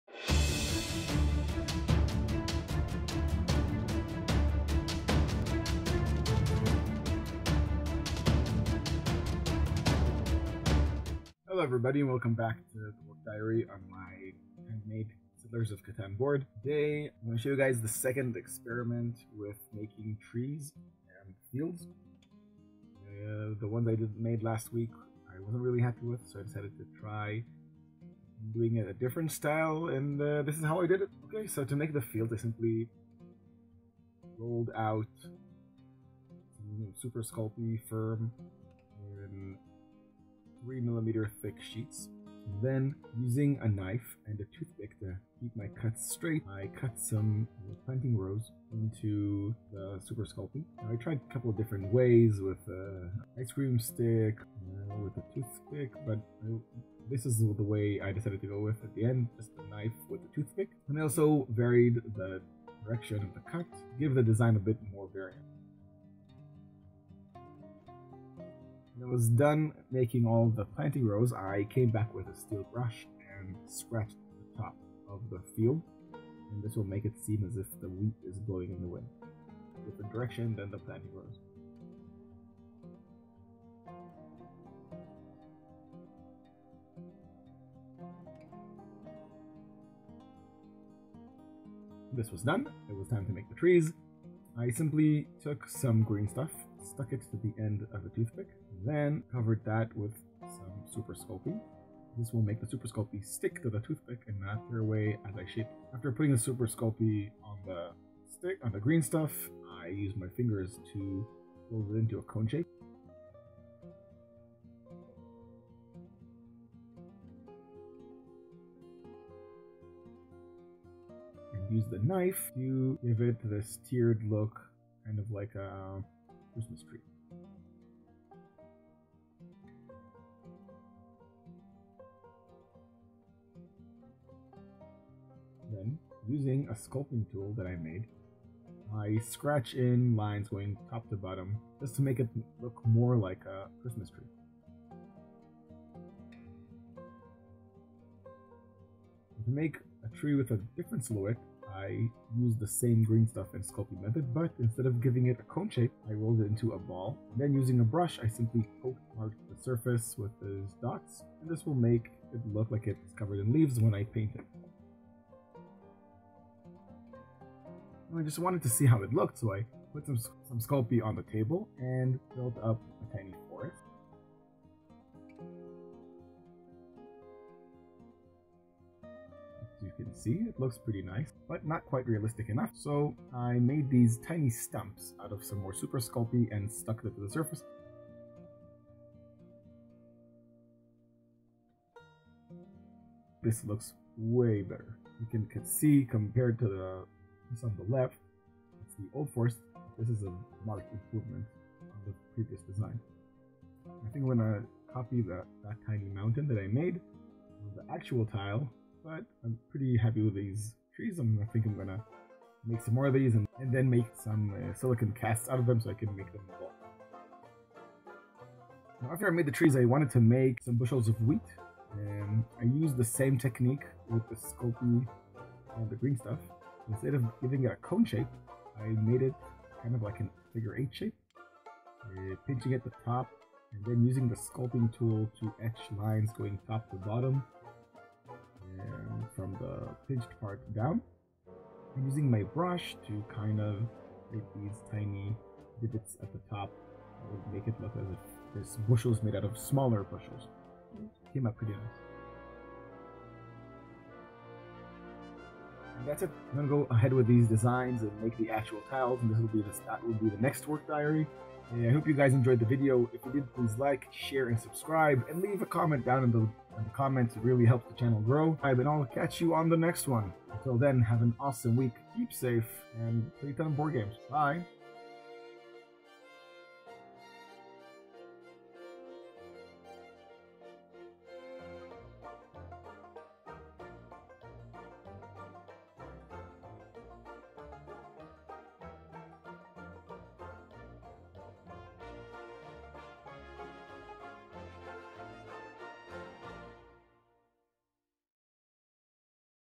Making a 3D settlers of Catan Game Board tutorial - Crafters log part 4 - forests of catan
In this installment of the work diary I take a second shot at sculpting trees and fields for my tiles. these make for a great tutorial for all of yo planning to craft your own boards and these are the designs i plan to move ahead with.

materials:
Greenstuff
Super Sculpy
Dass clay

I bought the greenstuff and super sculpy at greenstuff world

dungeoncrawler1985@gmail .com
Created with Movavi Video Editor Plus